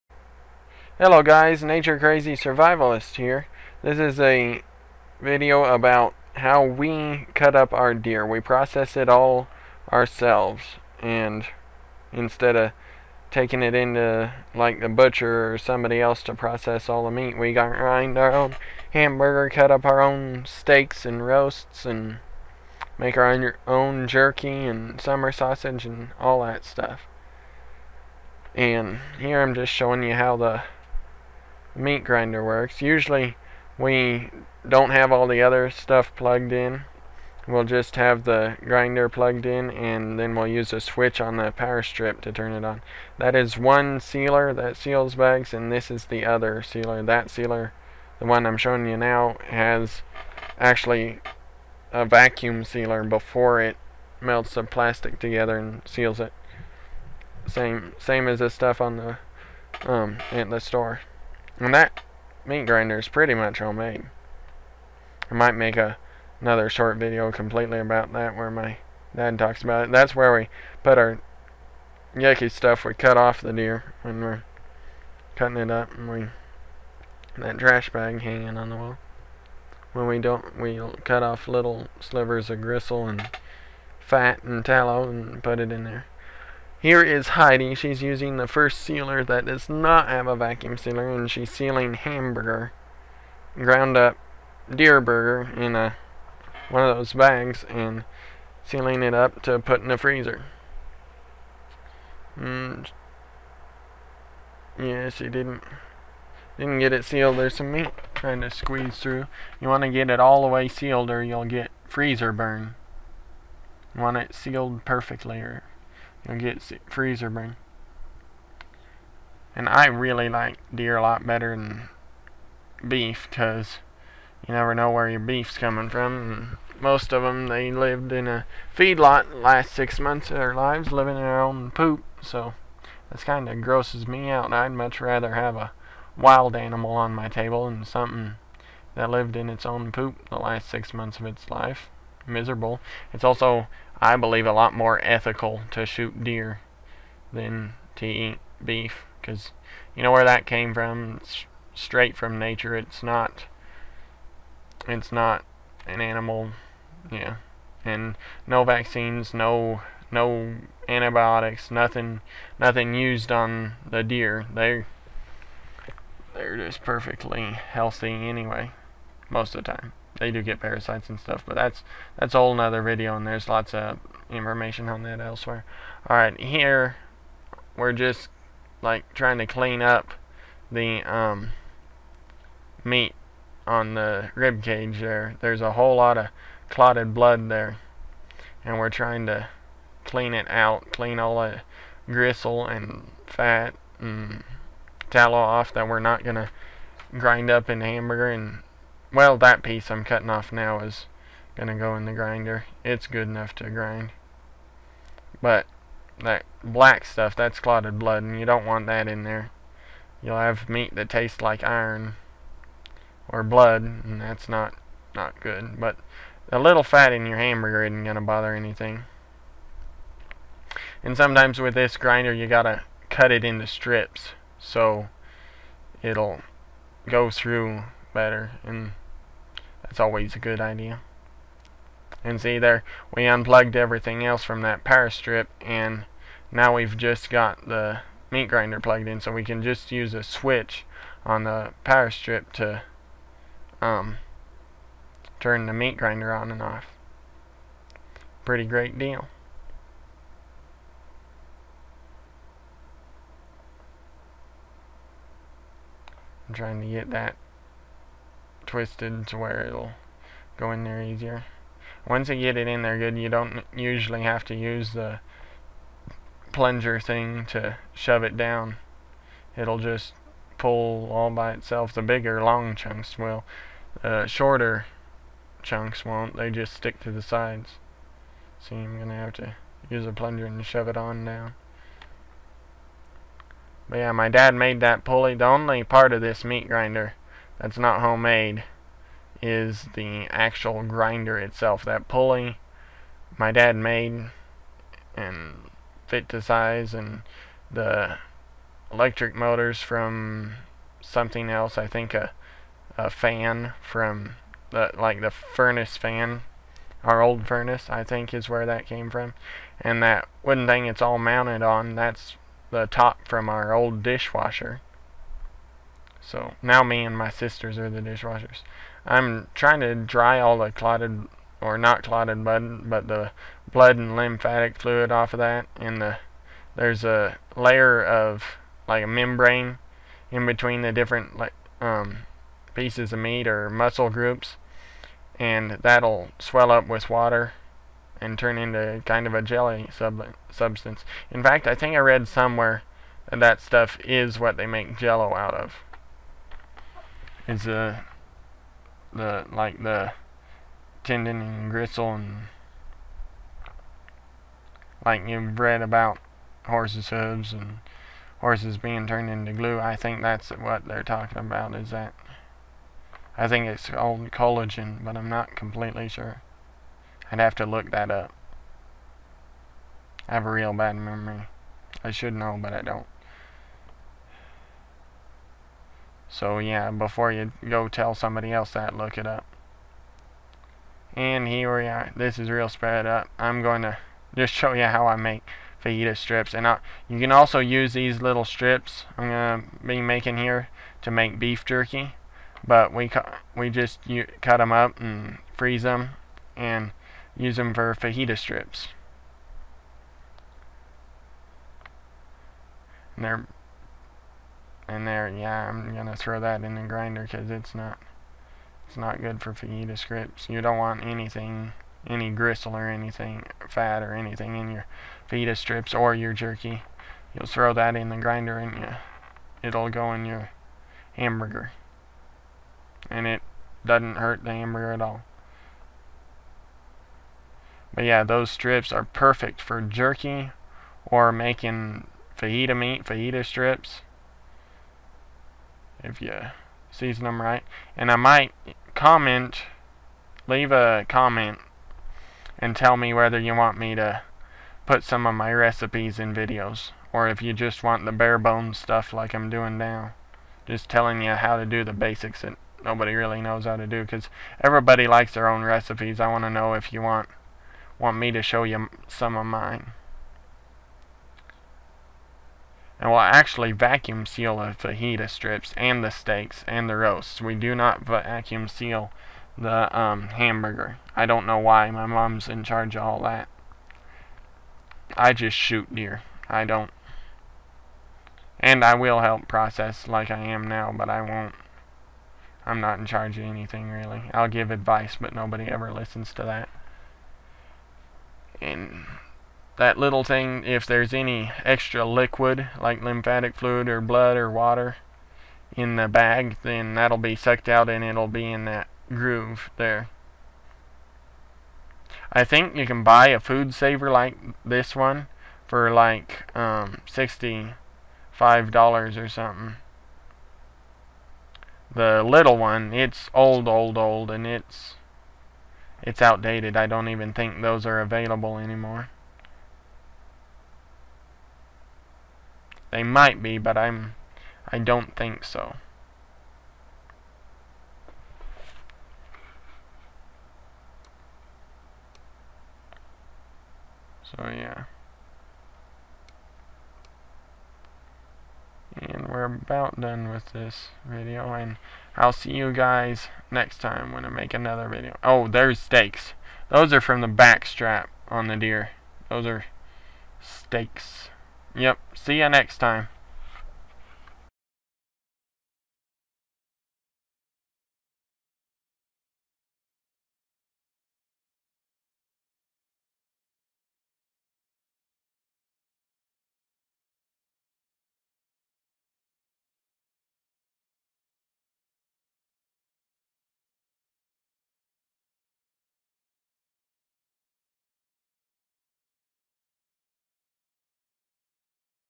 Processing Deer. How My Family Processes Our Deer Meat.
Graphic!

This is how we process our deer meat. We do it ourselves, so its free!! At least it is if you don't count the time spent. The way we do it is far from perfect, but we get more meat off than almost anybody.

This video is far from complete , but its what I was willing to try and fit in one small video.